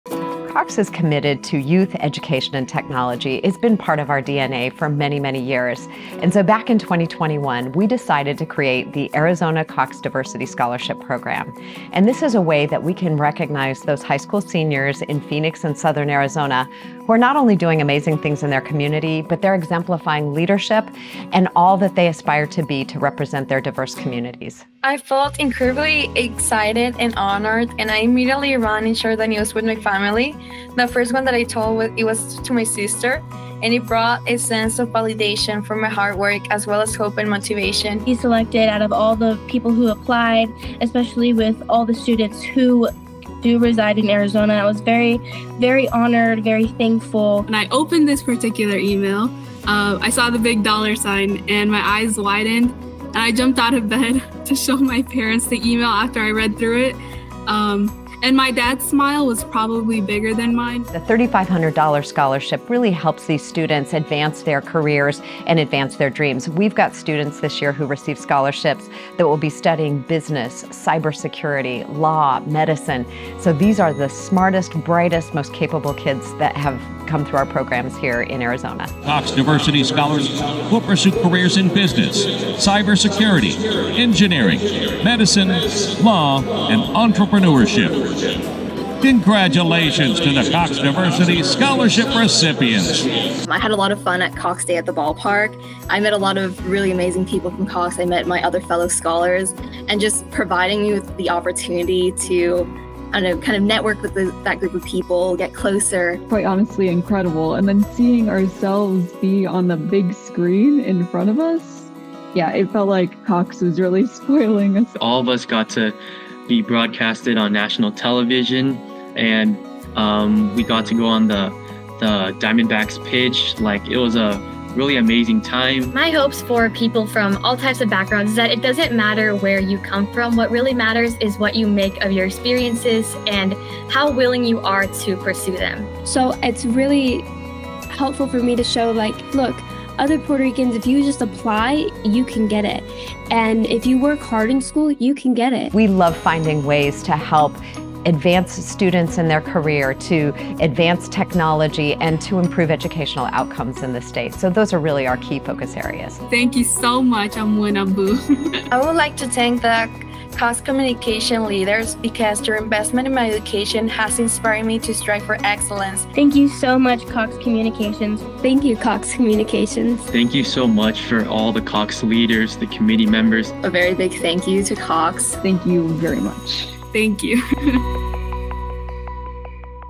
2024 Cox Diversity Scholarship Arizona Recipients
COX Awarded 10 amazing young students as Cox Diversity Scholarship Arizona recipients. Cox awarded $35,000 in scholarships to diverse Arizona high school grads. Each student received $3,500 as they prepare to enter college.